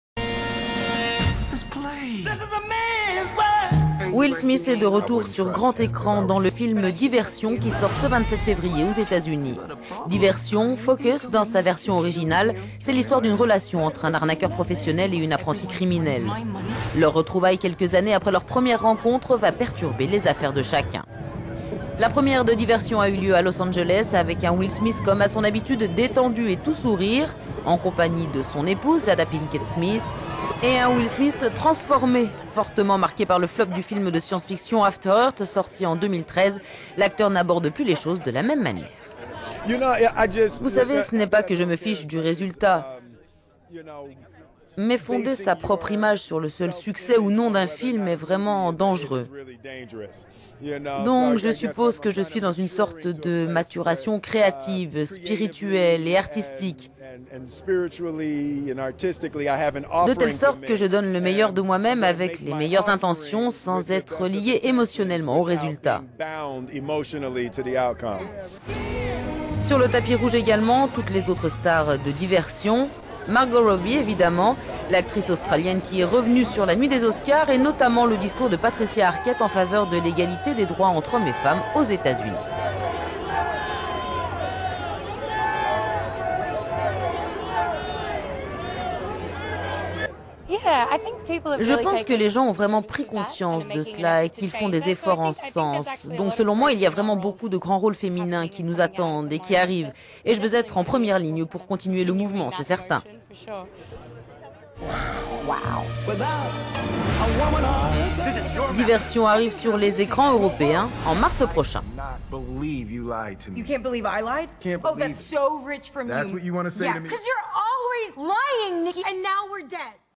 Will Smith tente la "Diversion" après le flop d'"After Earth" - cinema
Will Smith est de retour sur grand écran dans le film "Diversion" qui sort ce 27 février aux Etats-Unis. "Diversion", "Focus" dans sa version originale, raconte l'histoire d'une relation entre un arnaqueur professionnel et une apprentie criminelle. Leurs retrouvailles, quelques années après leur première rencontre viennent perturber les affaires de chacun.

La première de "Diversion" a eu lieu à Los Angeles avec un Will Smith comme à son habitude détendu et tout sourire et accompagné de son épou…